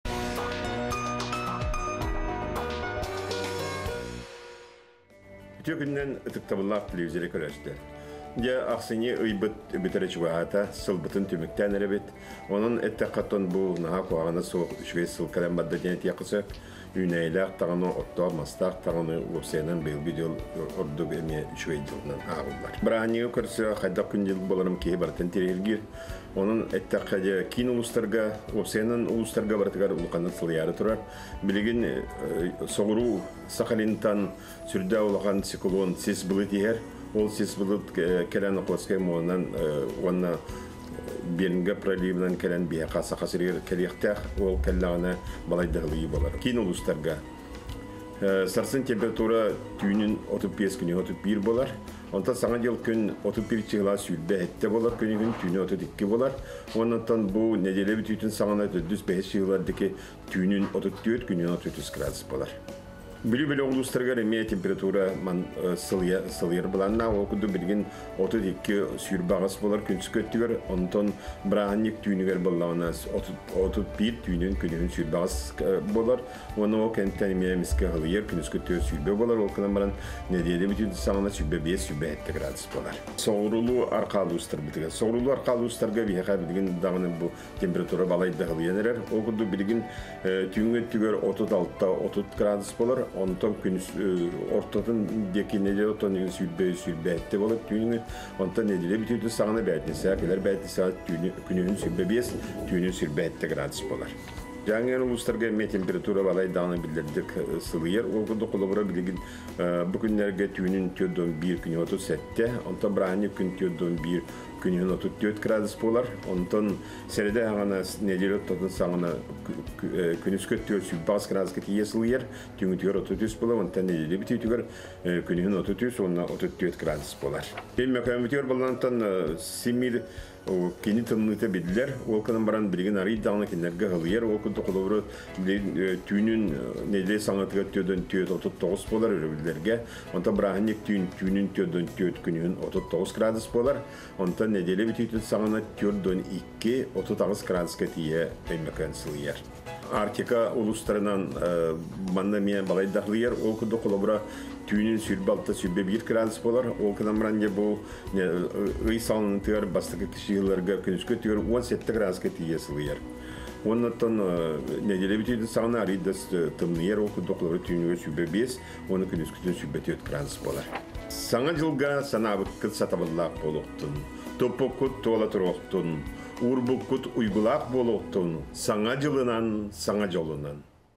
Саҥа дьыл күнүгэр халлаан сылыйыаҕа - Тускул
Официальный сайт NVK-ONLINE.RU - "Саҥа дьыл күнүгэр халлаан сылыйыаҕа", - диэн норуот билгэһитэ Тускул "Саха" НКИХ биэриитигэр эттэ. Бырааһынньык күннэригэр Саха сиригэр халлаан туругун сиһилии кэпсиирин истиҥ.